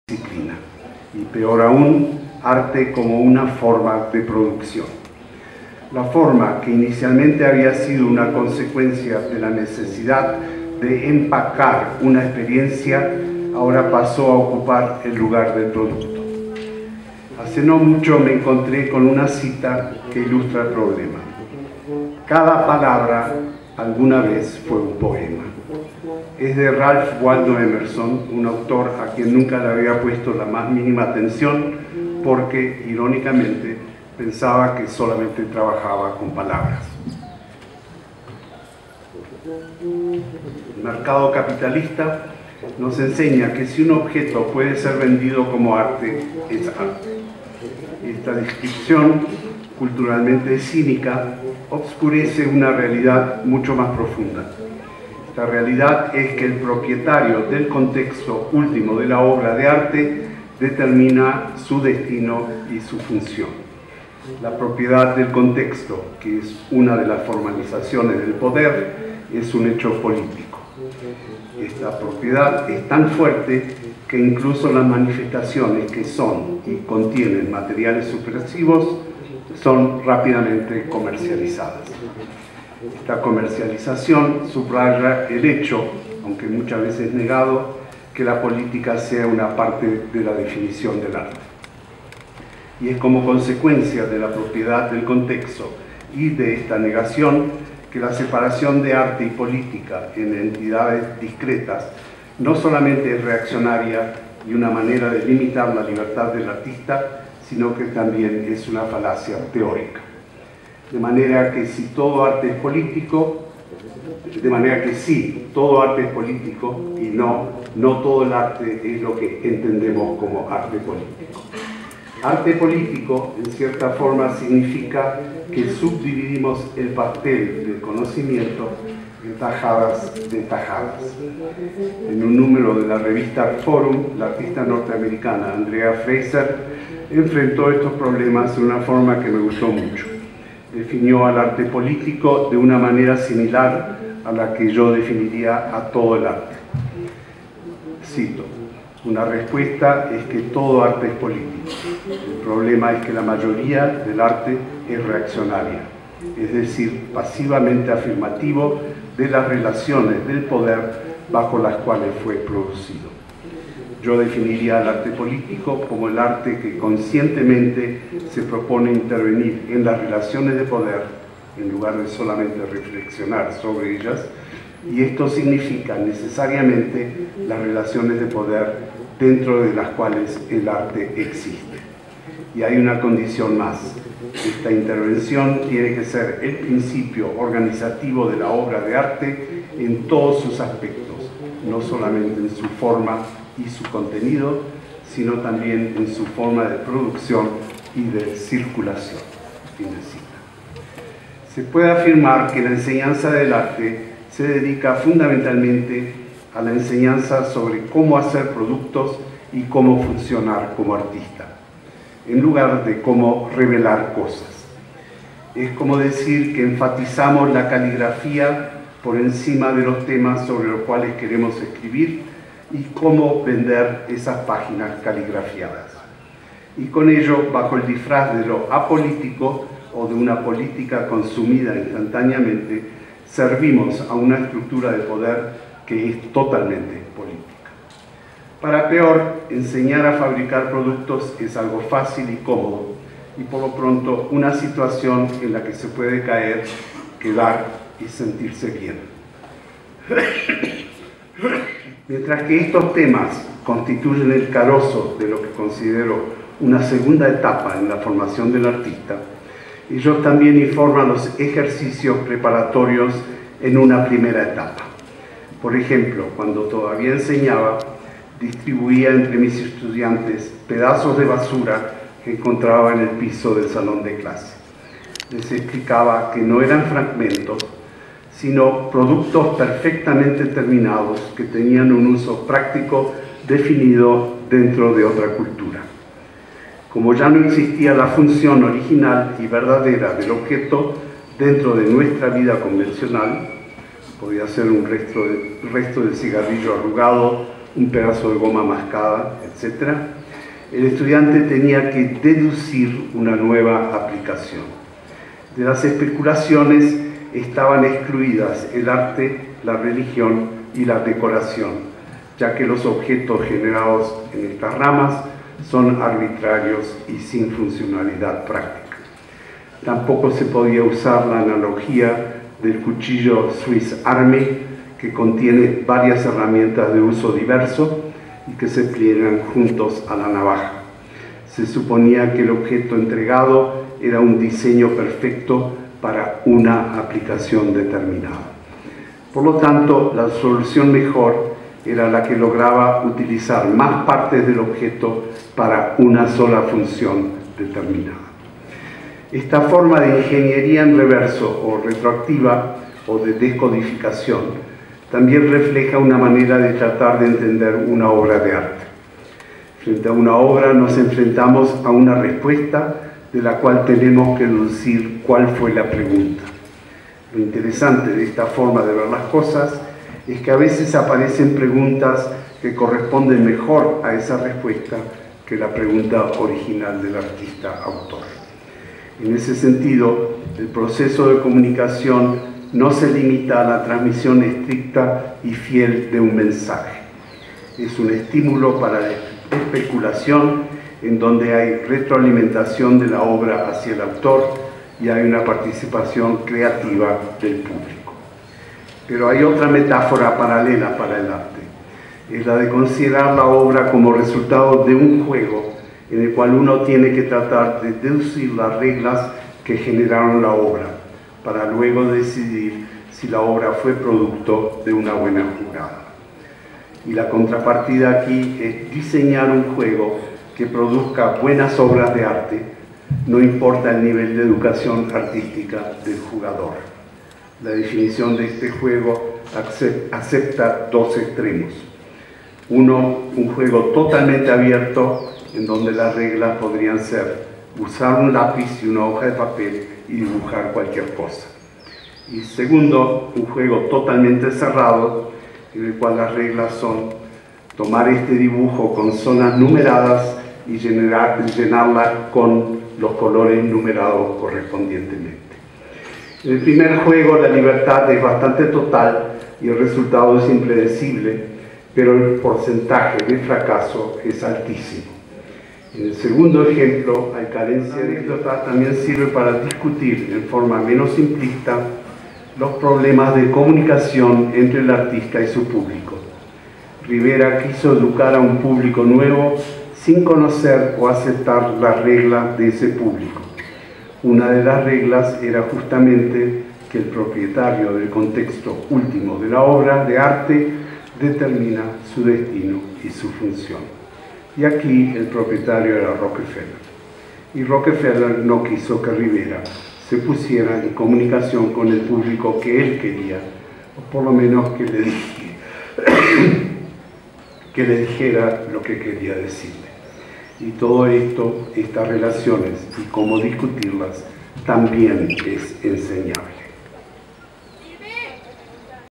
La enseñanza del arte como fraude
Luis Camnitzer, Artista y teórico en conversatorio con Katrin Steffen, Curadora de la Colección Daros Latinamerica, María Belén Sáez de Ibarra, Directora Nacional de Divulgación Cultural y curadora, y Alejandro Burgos,filósofo, escritor y curador

Miércoles 21 de marzo a las 3:00 p.m.
Sala de conferencias, Auditorio León de Greiff